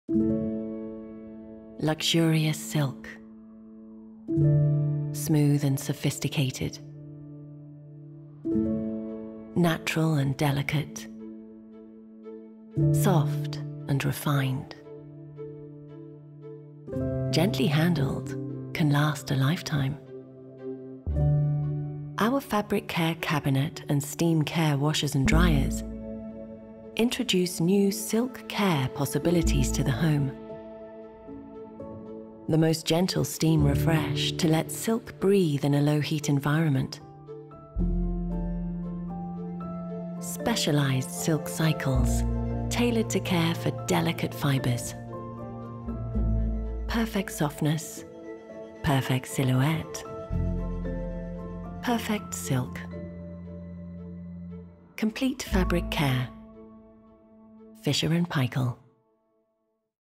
Fabric Care: Perfect Silk
From delicate scarves, camisoles and robes to the beautiful drape of a timeless silk shirt, silk is naturally luxurious and elegant. From the gentlest steam refresh to intelligent wash cycles tailored to the qualities of this ultra-delicate fibre, our Fabric Care Cabinet and Steam Care washers and dryers evolve your repertoire of silk care possibilities at home.

Perfect drape, perfect lustre, perfect silk.

Discover more Fabric Care Insights: [link]